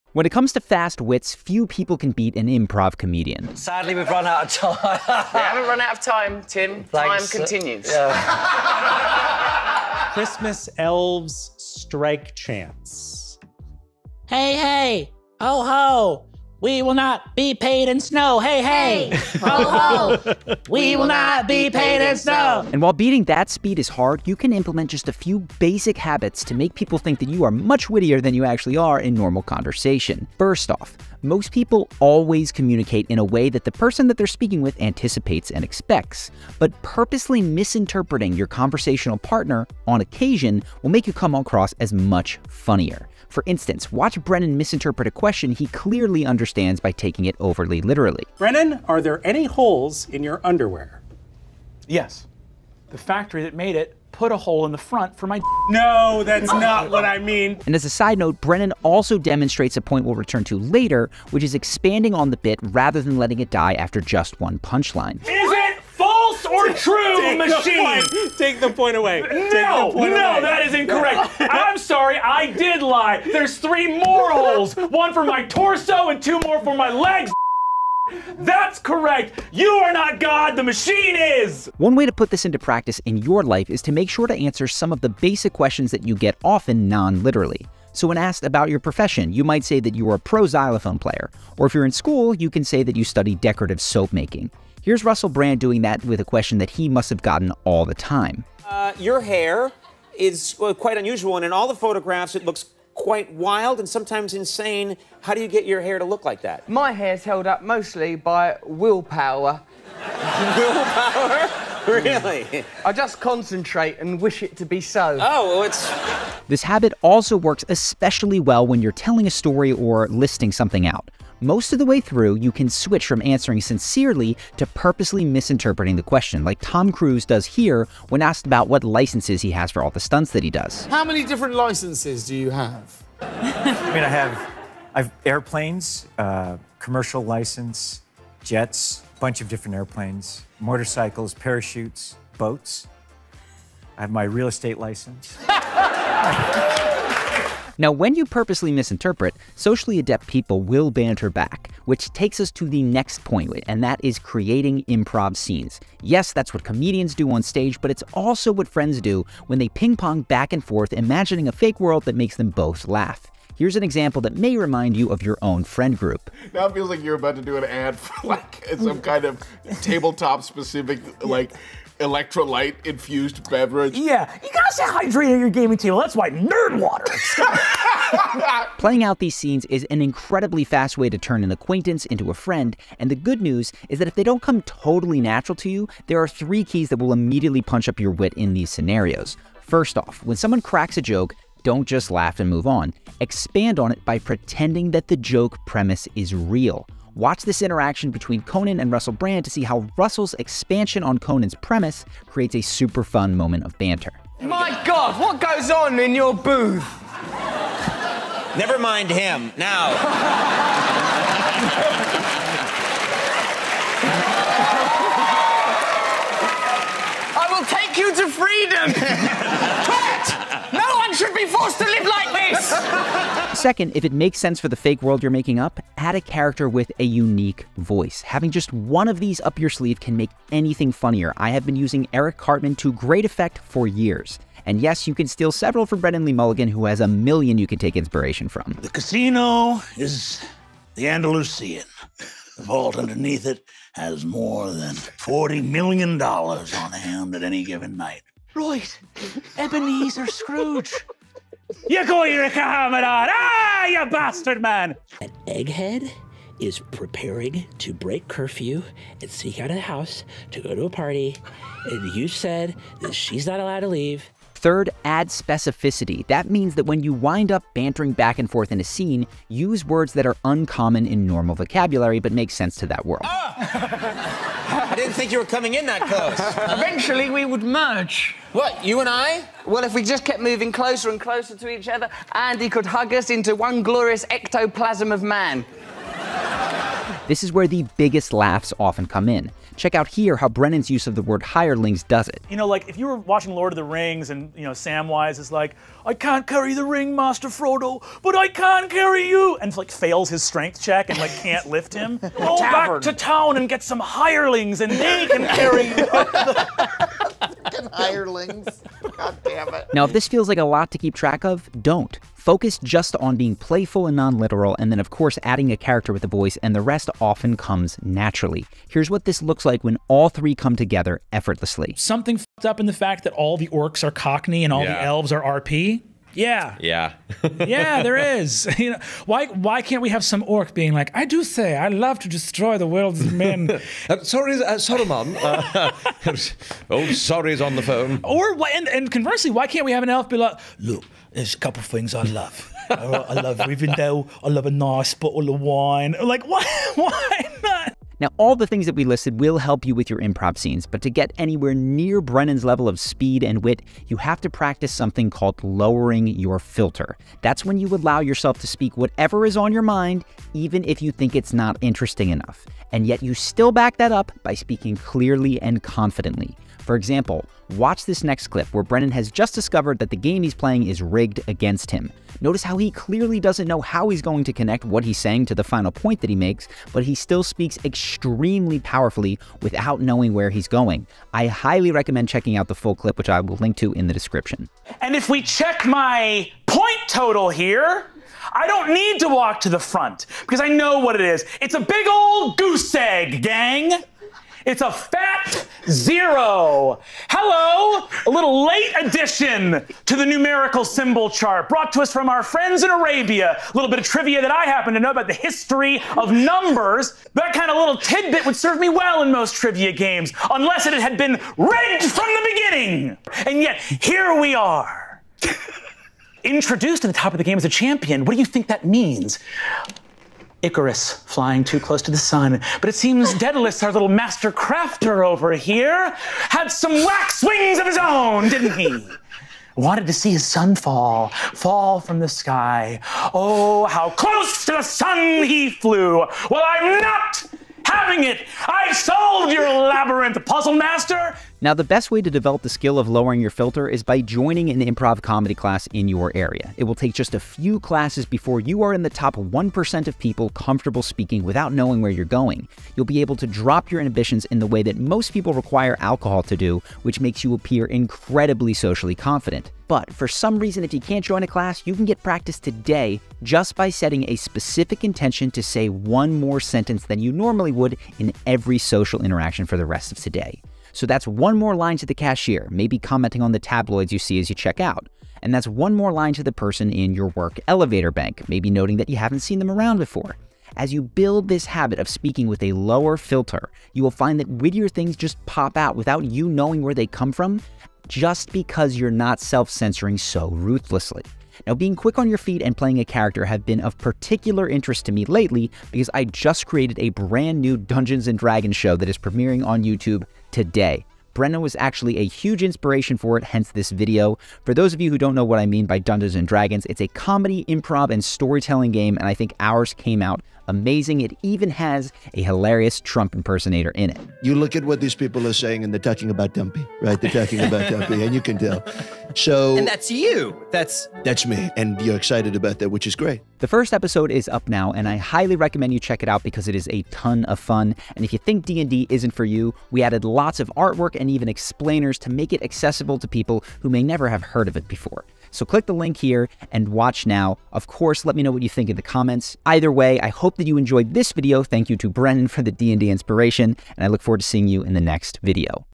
How To Always Have Something Funny To Say
Improv comedians have some of the fastest wit.

Getting to that speed is tough, but you can implement just a few basic habits to make people think you’re much wittier in normal conversation.

⏰TIMESTAMPS⏰

0:00 - Intro
0:36 - #1: Purposely misinterpret
2:31 - #2: Create improv scenes
5:47 - #3: Lower your filter